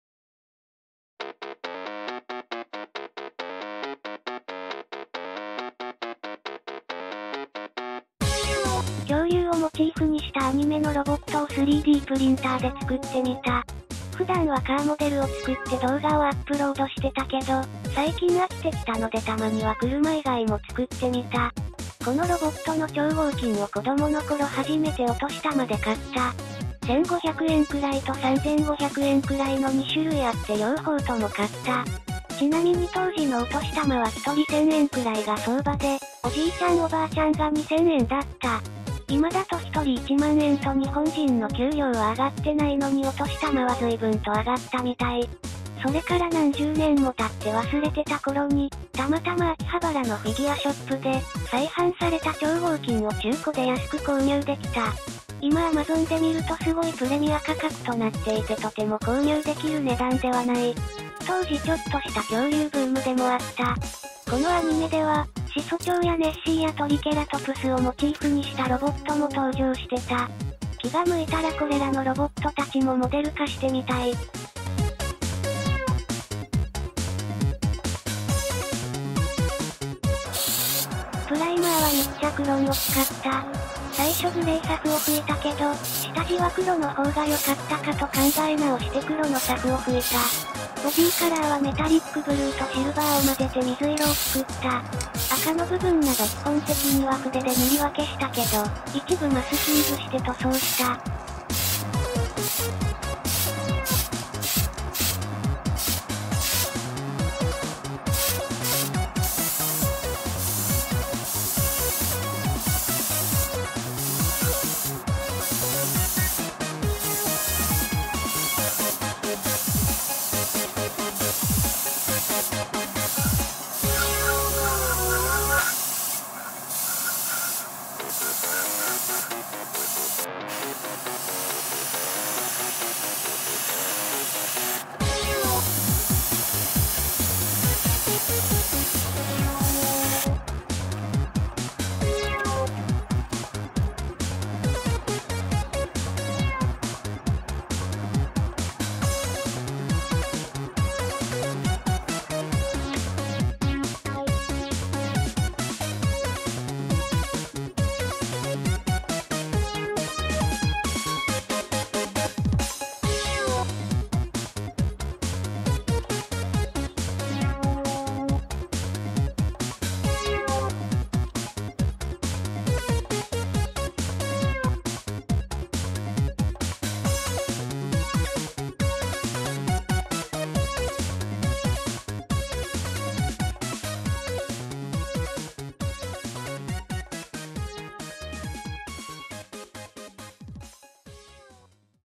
超合金を3Dプリンターで作る。
子供のころ初めてのお年玉で買った超合金を3Dプリンターで再現してみた。動画の背景に写っているのは超合金魂というのちに再版されたもののようです（中古で買ったので詳細は不明）。ガイキングのプラモデルも最近グッドスマイルカンパニーから再版されたようです。

超合金とは、
ロボットアニメ・特撮作品のダイキャスト製キャラクター玩具のシリーズブランド名、つまりミニカーのロボット版？

Design tool: Blender
3D printer: Anycubic Photon
Resin: Wanhao water-washable resin

3D data
Scale 1/4000